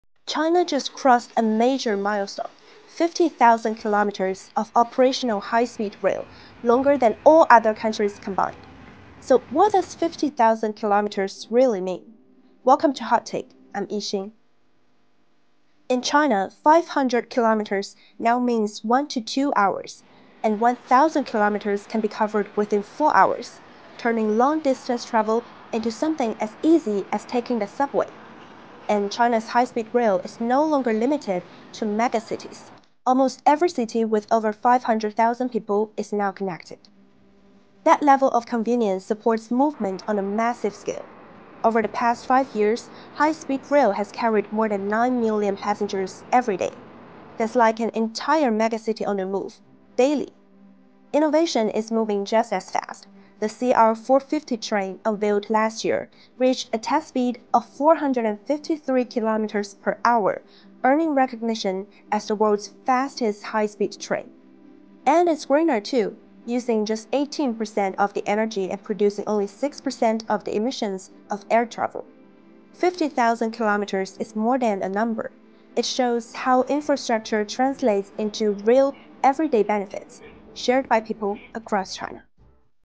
What does 50,000 kilometers of high-speed rail really mean?
China's high-speed rail network has just passed 50,000 kilometers—the longest in the world. Beyond the headline number, what does that actually mean for everyday travel and daily life? Here’s how high-speed rail is reshaping how people move and live across China.